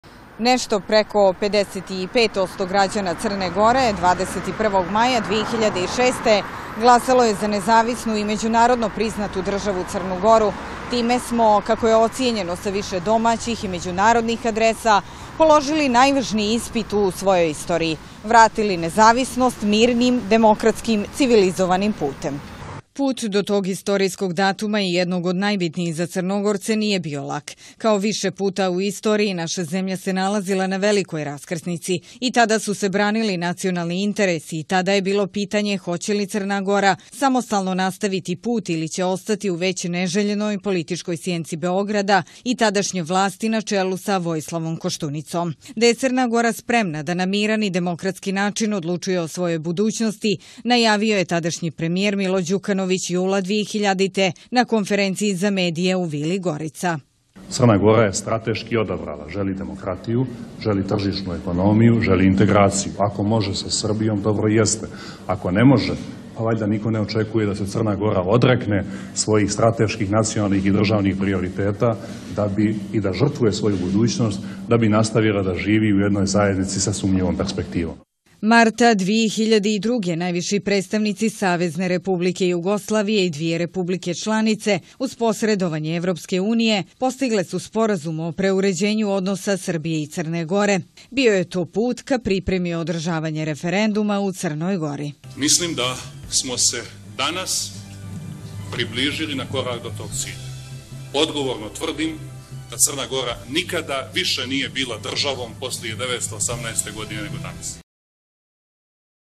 NOVA M - CRNA GORA PRIJE I POSLIJE REFERENDUMA 21. MAJA 2006
Četrnaest je godina od obnove crnogorske nezavisnosti. Na referendumu 21. maja 2006. godine, 55,5% građana naše zemlje dalo je glas za nezavisnu Crnu Goru sa punim međunarodnim i pravnim subjektivitetom. Kako je izgledao put do istorijskog datuma – 21. maja, te šta je prethodilo referendumu, a uslijedilo nakon toga pogledajte u osvrtu Ivane Drobnjak.